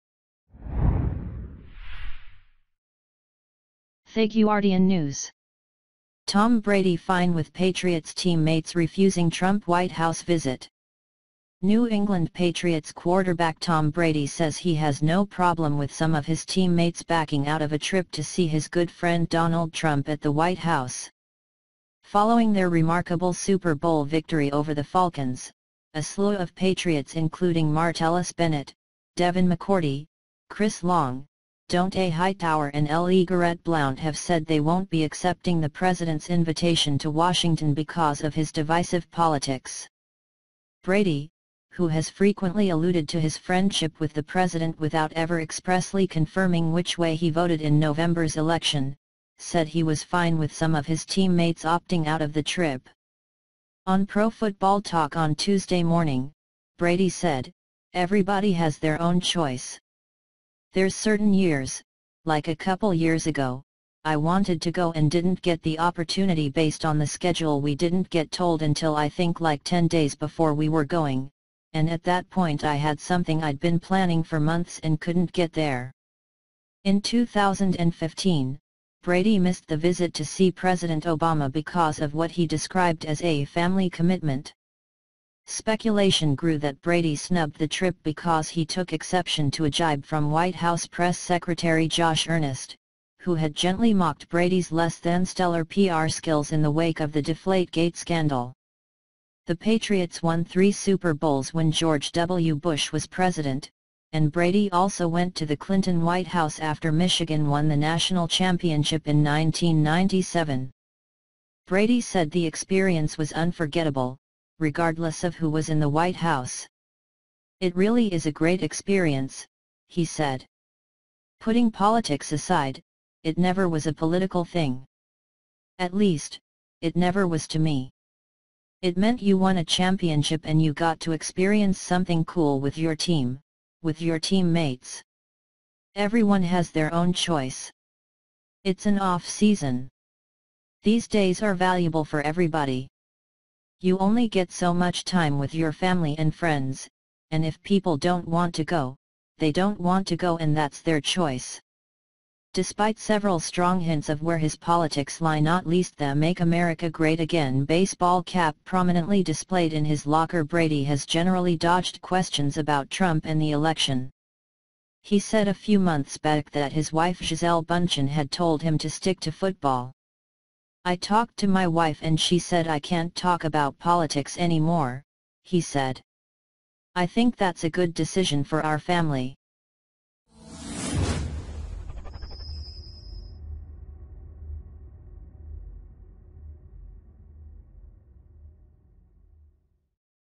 Tom Brady fine with Patriots team mates refusing Trump White House visit
Sources:

NaturalNews.com
ForeignPolicy.com
Breitbart.com
TheNationalSentinel.com
GoodGopher.com